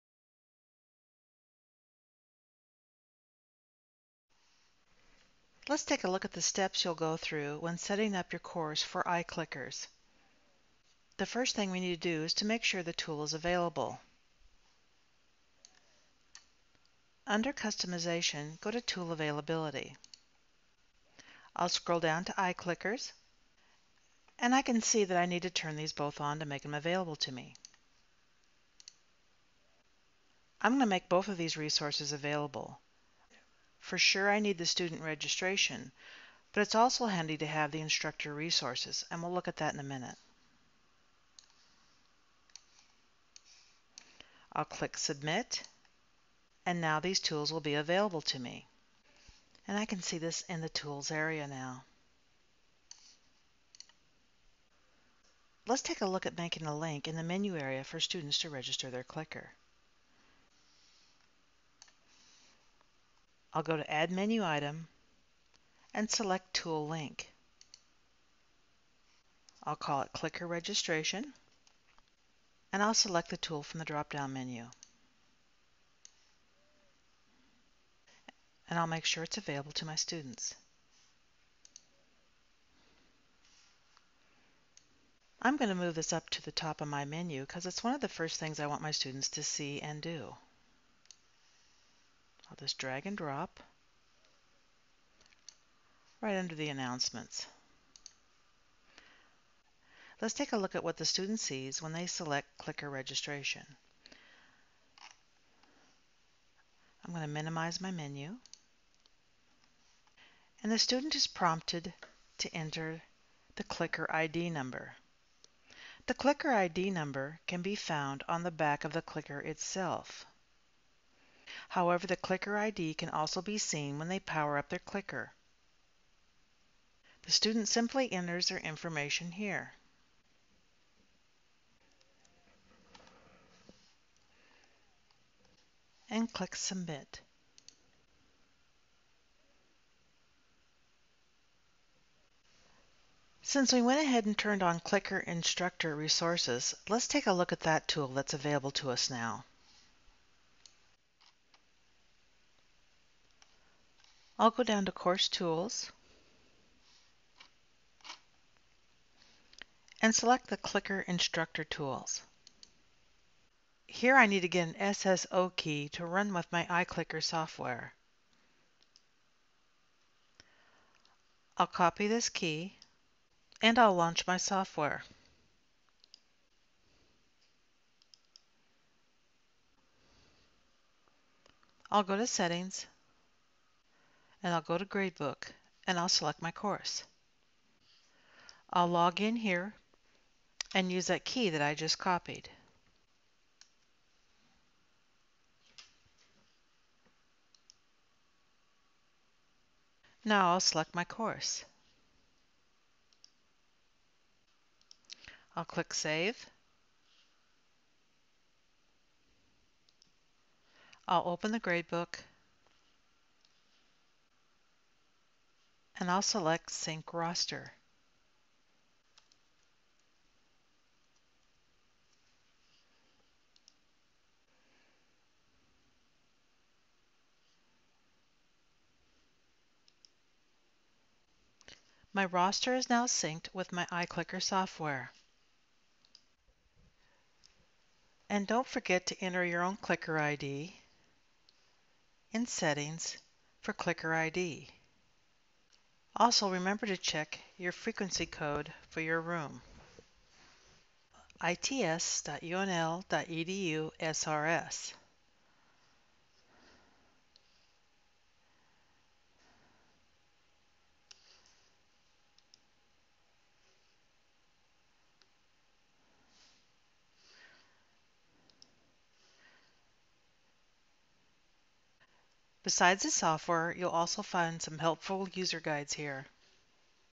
iClicker registration in Blackboard
How to setup and register your iClicker in Blackboard